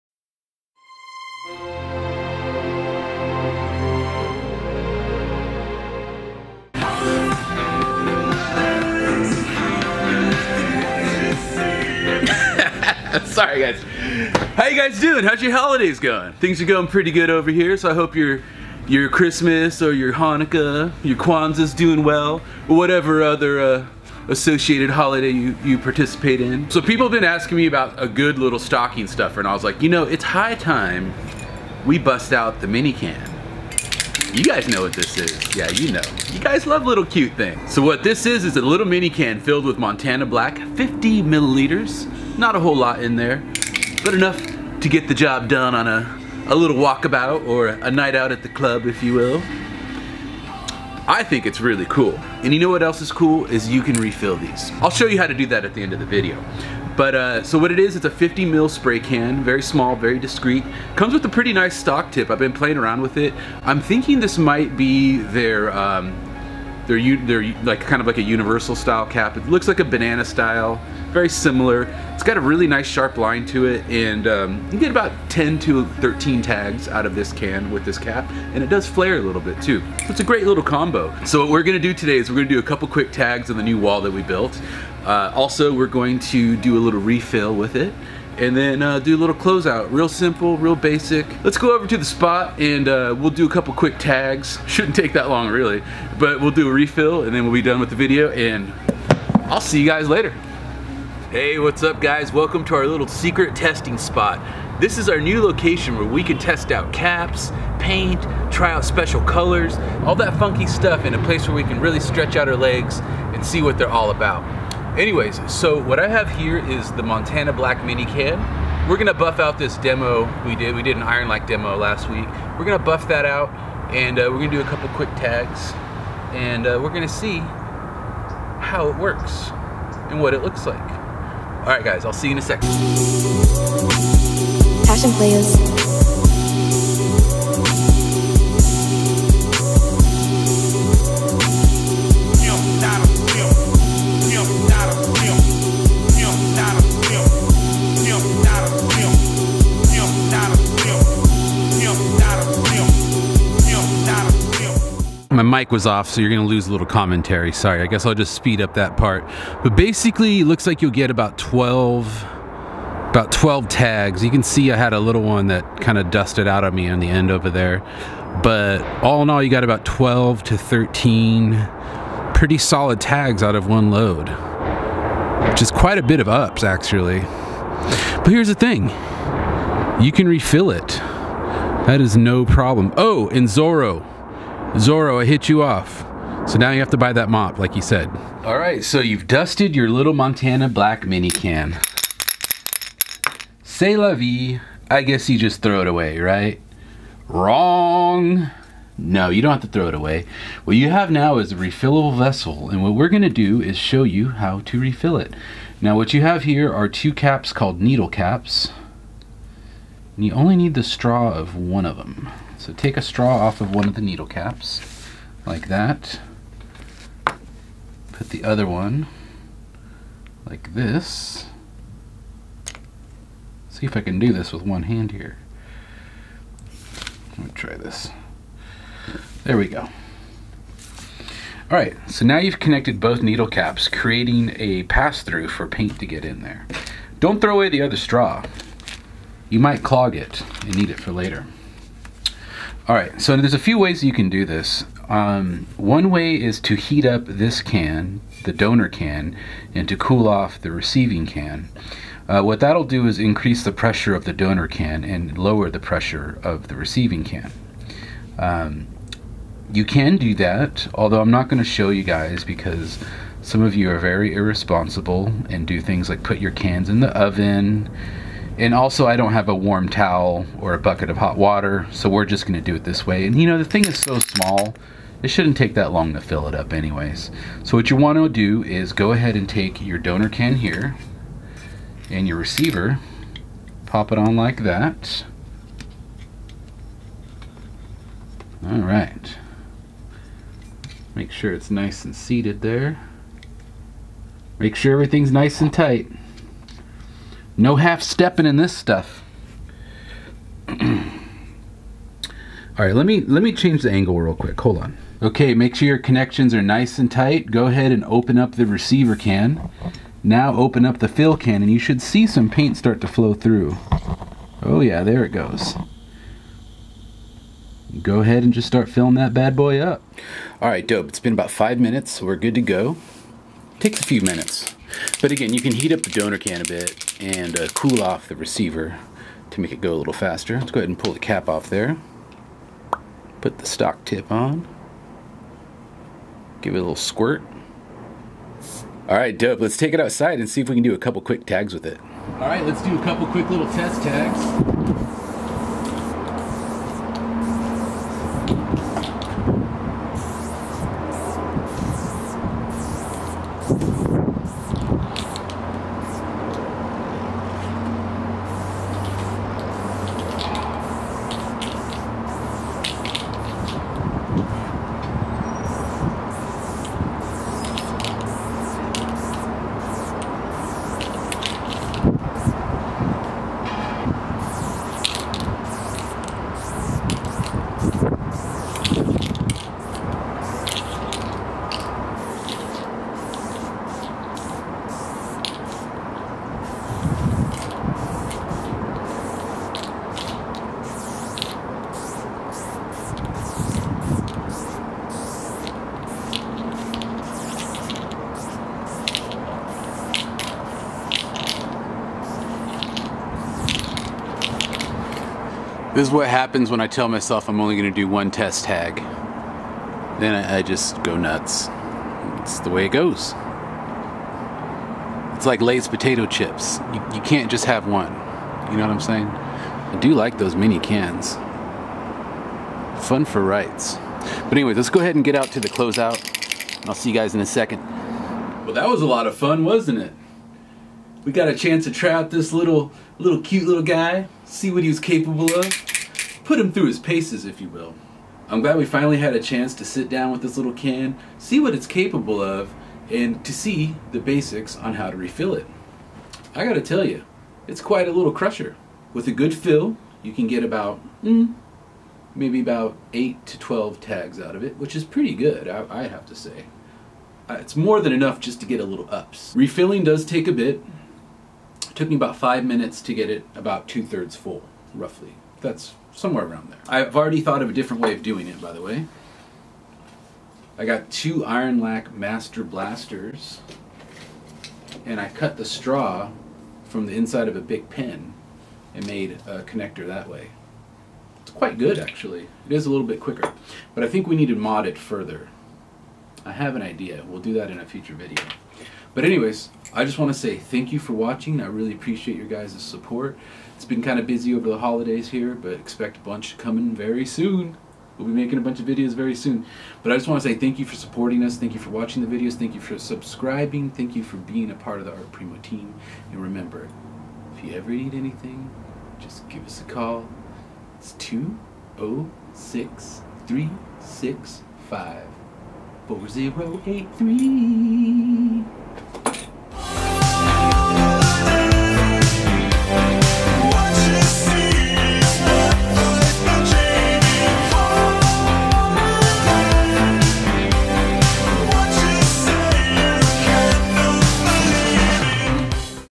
Montana Black 50ml Mini Can Review
In this video, GR tries out the Montana mini can. He also shows you how to refill it.